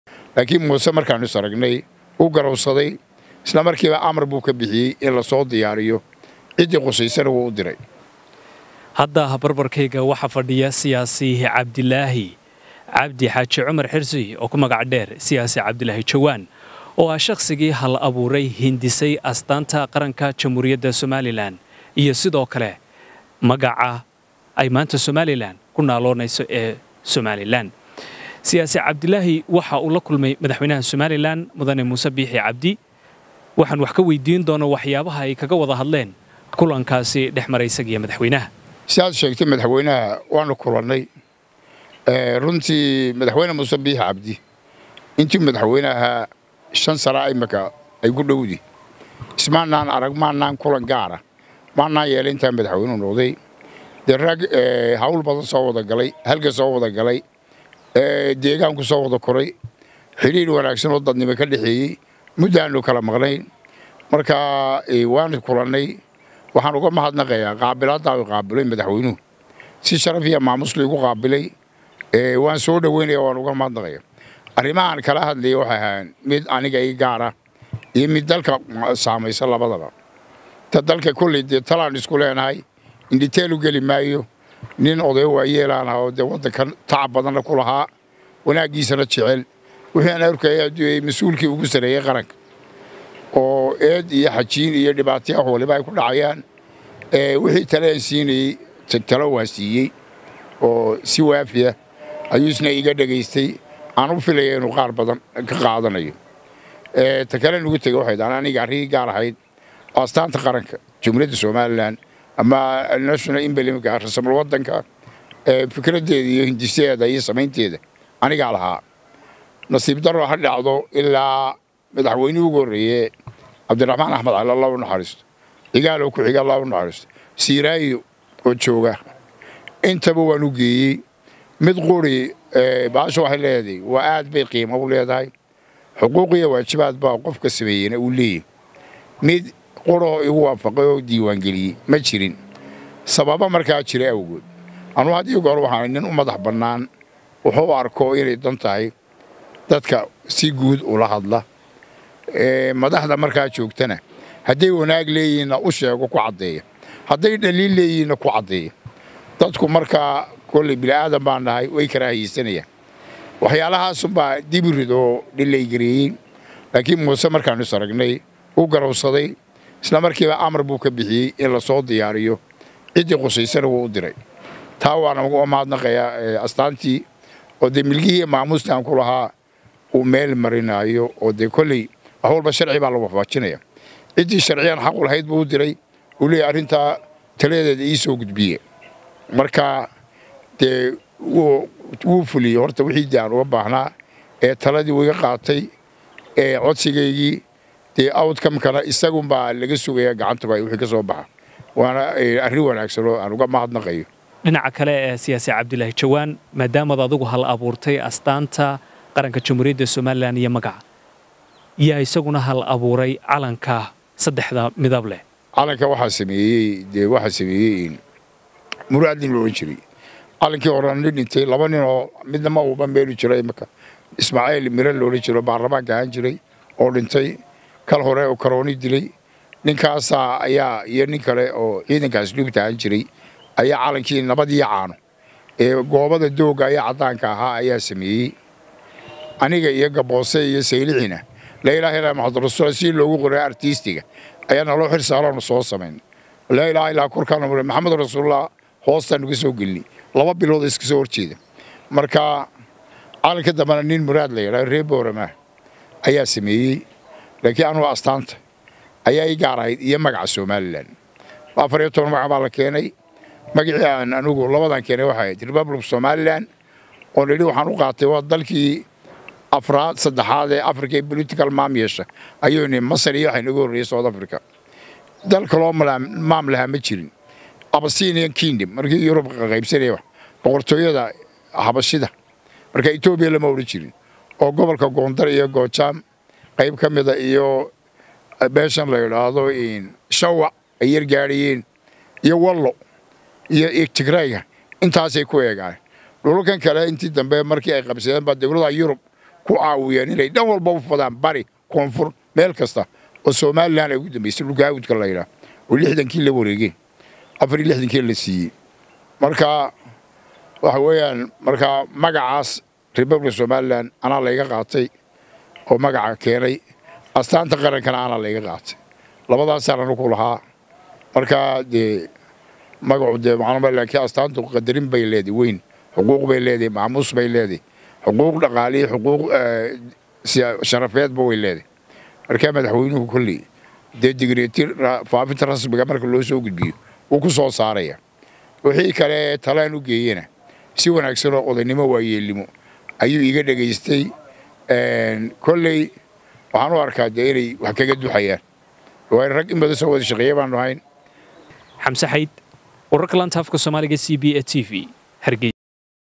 Siyaasi cabdilaahi jawaan ayaa ka waramay kulan uu la qaatay madaxweynaha Somaliland!...
Siyaasi cabdilaahi jawaan ayaa ka waramay kulan uu la qaatay madaxweynaha Somaliland oo ku saabsana talo wadaag xaalada dalka ah.